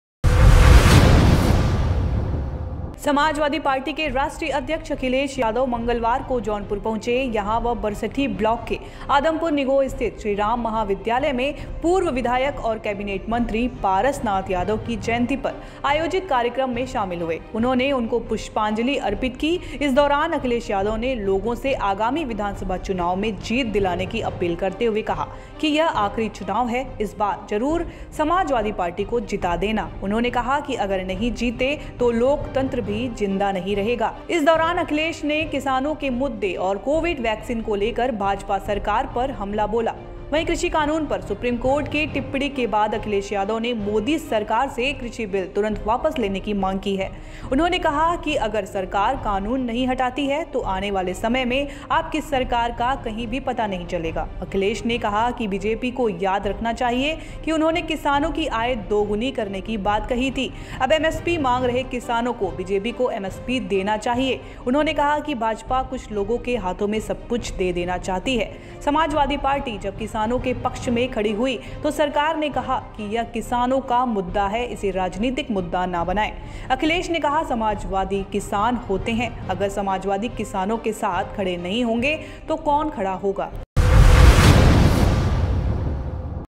कृषि बिल और कोरोना वैक्‍सीन को लेकर अखि‍लेश का BJP पर हमला # The Journalist News
कृषि कानून पर सुप्रीम कोर्ट की टिप्पणी के बाद अखिलेश यादव ने मोदी सरकार से कृषि बिल तुरंत वापस लेने की मांग की है। उन्होंने कहा कि अगर सरकार कानून नहीं हटाती है तो आने वाले समय में आपकी सरकार का कहीं भी पता नहीं चलेगा। अखि‍लेश ने कहा कि बीजेपी को याद रखना चाहिए कि उन्होंने किसानों की आय दोगुनी करने की बात कही थी। अब एमएसपी मांग रहे किसानों को बीजेपी को एमएसपी देना चाहिए। उन्‍होंने कहा कि भाजपा कुछ लोगों के हाथों में सब कुछ दे देना चाहती है। समाजवादी पार्टी जब किसानों के पक्ष में खड़ी हुई तो सरकार ने कहा कि यह किसानों का मुद्दा है, इसे राजनीतिक मुद्दा न बनाएं।